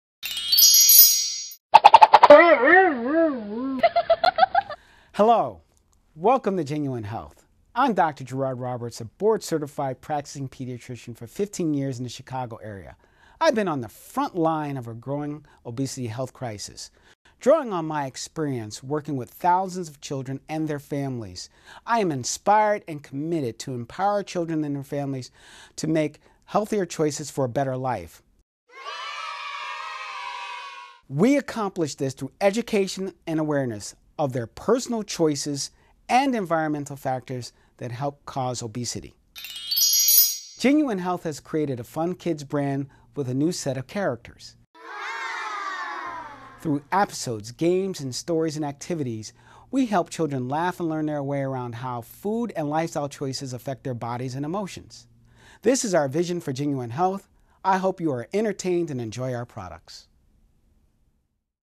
Dr. Gerard Roberts On GenUwin Health's Mission
Dr. Gerard Roberts, Founder, GenUwin Health
GenUwin Health has created a fun kids brand with a cast of new characters. Through Stories, appisodes, games and activities, we make kids laugh and learn their way around how food and lifestyle choices affect their bodies and emotions.